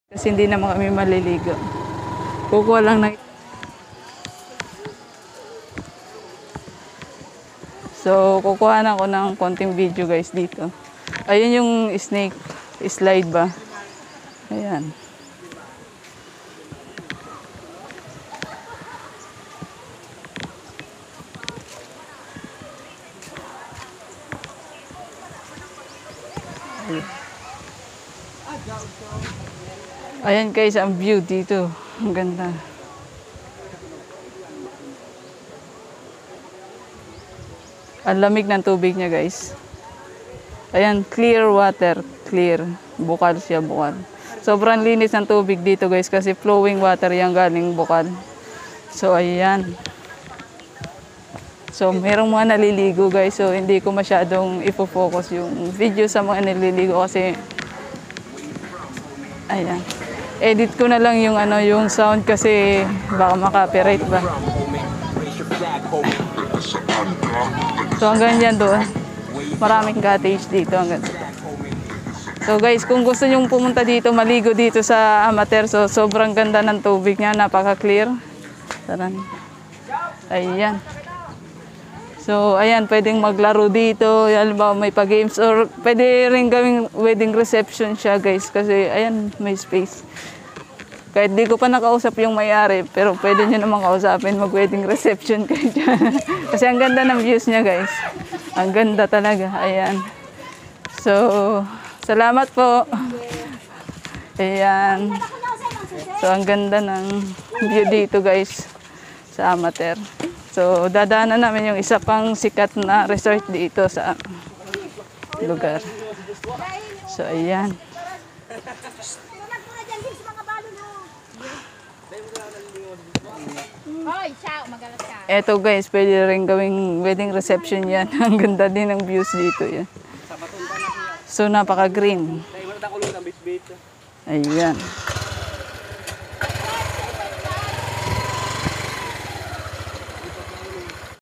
PASILIP SA NAPAKAGANDANG RESORT SA MAY BAKURAN NAMIN AMATER SPRING RESORT
MY YOUTUBE CHANEL THANK YOU 😘😘😘